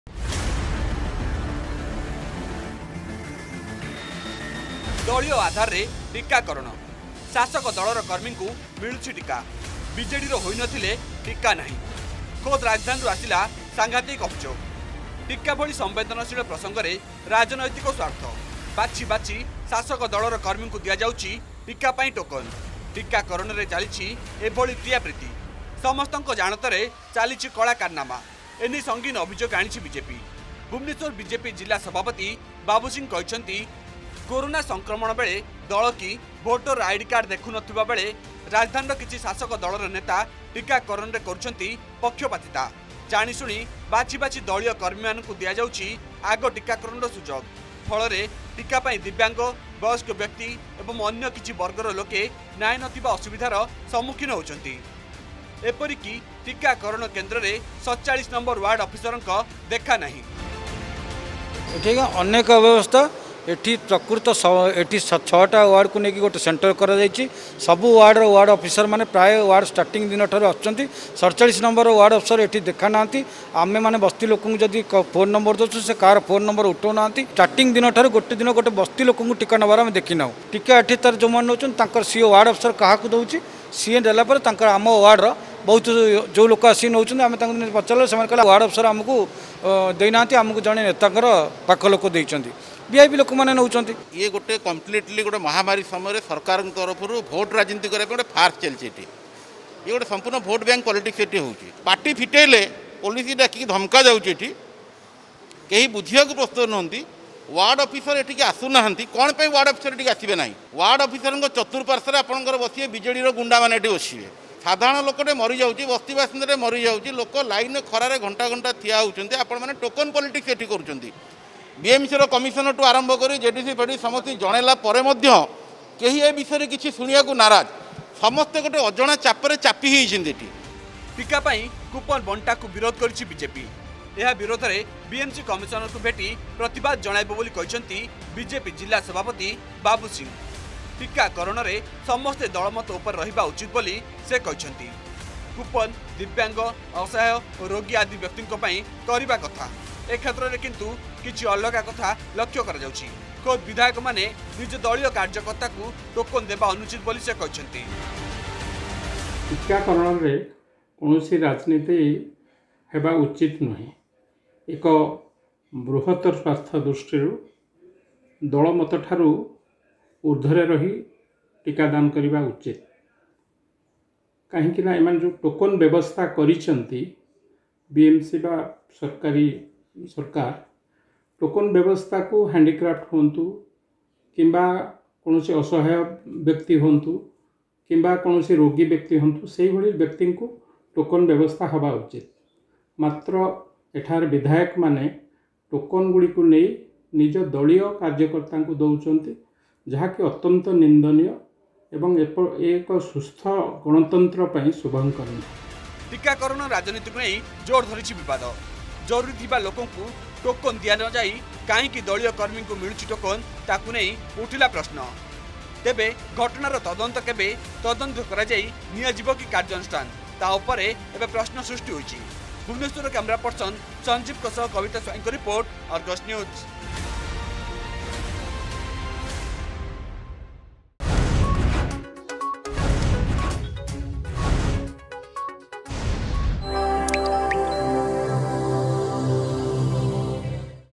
ଟିକାକରଣ ଟୋକନ୍ ବଣ୍ଟା ହେଉଛି ରାଜନୀତି ଦଳ ଭିତ୍ତିରେ
ଟିକା ଭଳି ସମ୍ବେଦନଶୀଳ ପ୍ରସଙ୍ଗରେ ରାଜନୀତିକ ସ୍ୱାର୍ଥ ।ବାଛି ବାଛି କର୍ମୀମାନଙ୍କୁ ଦିଆଯାଉଛି ଟିକା ପାଇଁ ଟୋକନ୍।ଖୋଦ୍ ରାଜଧାନୀ ଛାତି ଉପରେ ଚାଲିଛି ଏଭଳି ପ୍ରିୟା ପ୍ରୀତି।  ଏଭଳି ଅଭିଯୋଗ ଆଣିଛନ୍ତି ଭୁବନେଶ୍ୱର ବିଜେପି ଜିଲ୍ଲା ସଭାପତି ବାବୁ ସିଂ...